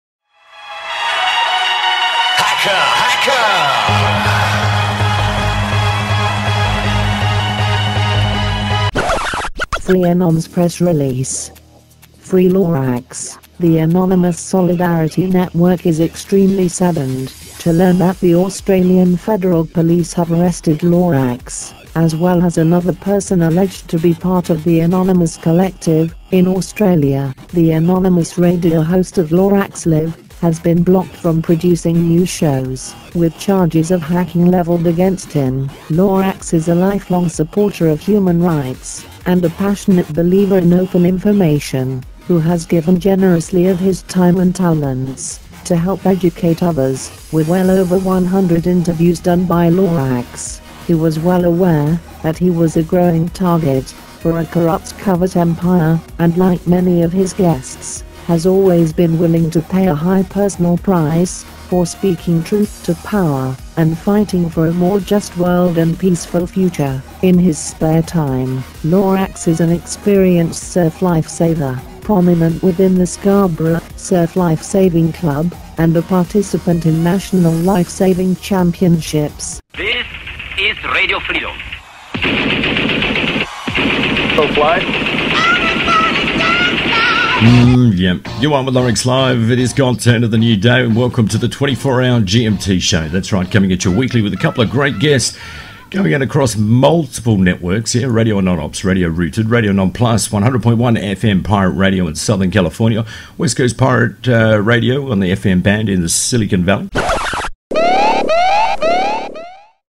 Anonymous - Free Lorax
(This is NOT a GHOPS original, please see attribute.)

Askari Hatari

FreeAnons Press Release. Free Lorax.
22 May 2014 - Sad day! Lorax was arrested in Australia.

The Anonymous Solidarity Network is extremely saddened to learn that The Australian Federal Police have arrested Lorax, as well as another person alleged to be part of the Anonymous collective, in Australia.

The Anonymous radio host of Lorax Live has been blocked from producing new shows with charges of hacking leveled against him. Lorax is a life-long supporter of human rights and a passionate believer in open information who has given generously of his time and talents to help educate others.

With well over 100 interviews done by Lorax, he was well aware that he was a growing target for a corrupt covert empire and like many of his guests has always been willing to pay a high personal price for speaking truth to power and fighting for a more just world and peaceful future.